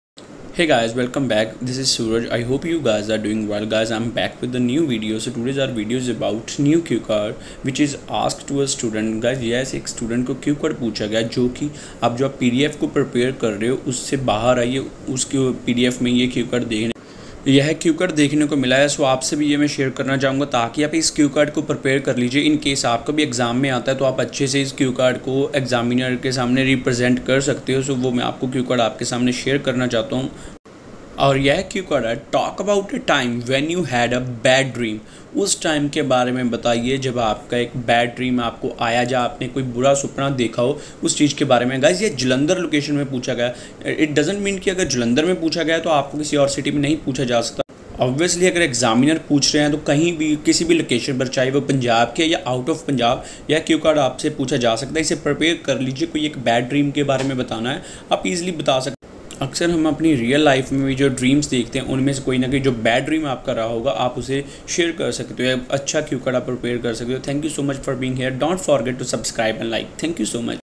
CUE CARDS CHANGED BEFORE AUGUST?? NEW CUE CARD ASKED TO STUDENT || IELTS WITH SURAJ
Listening Tips Video for 7.5+ Bands

Reading Tips video for 7.0+ Bands

TRUE FALSE  NOT GIVEN TIPS

Speaking Tips Video for 7.0+ Bands